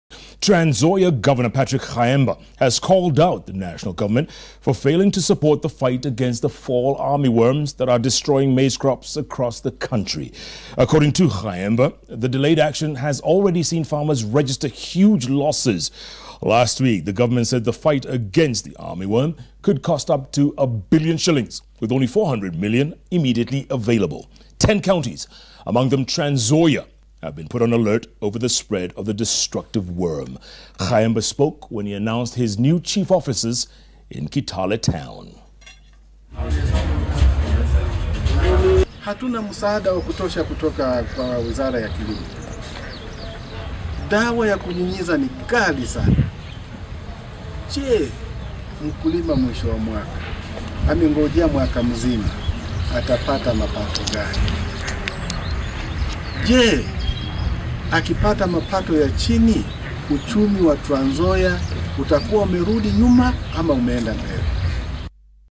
Governor Khaemba blames state for slow action on armyworm
Trans Nzoia governor Patrick Khaemba has called out the national government for failing to support the fight against the fall army worms that are destroying maize crops across the county.